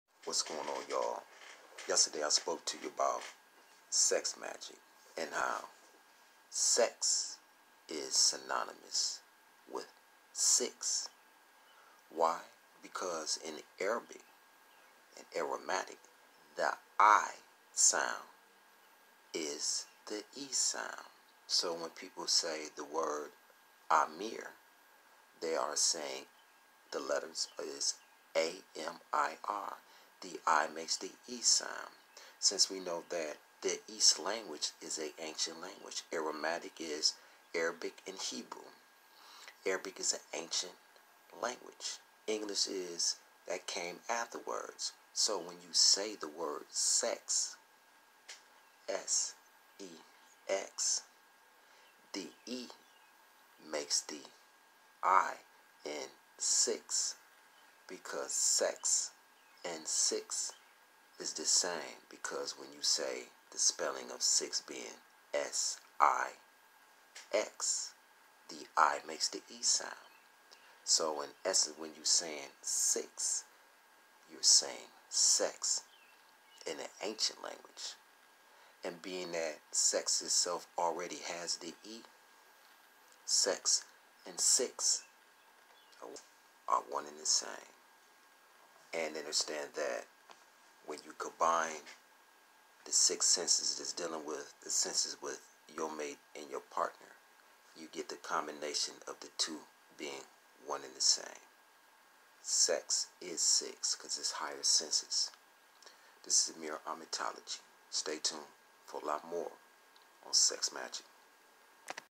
SEX💜MAGIK🔥 prt2. By. Amir Amøn L.
SEX💜MAGIK🔥 prt2. I explain the CONNECTION TO " SEX and SIX "AND. tune in👀

Follow me i.g. herb_specialist_513
For Health tips and wellness

I.g. amir_amon_l